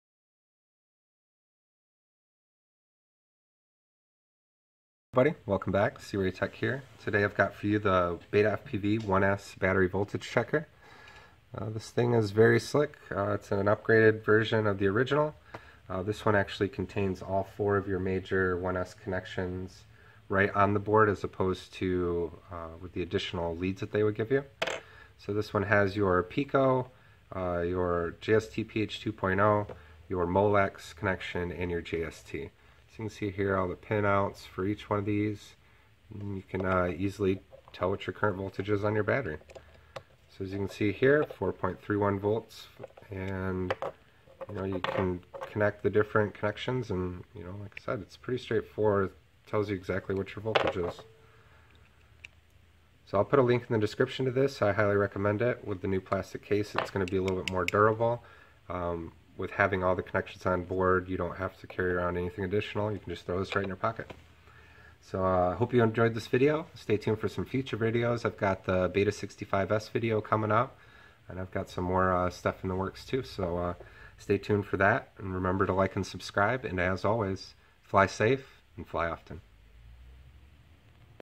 BetaFPV 1S Battery Voltage Tester
Today I have the BetaFPV 1s Battery Voltage Tester.  This is a great addition to anyone's tiny whoop or micro quadcopter arsenal.  Its small and does the trick.  You can see your current voltage anywhere.

Link to buy one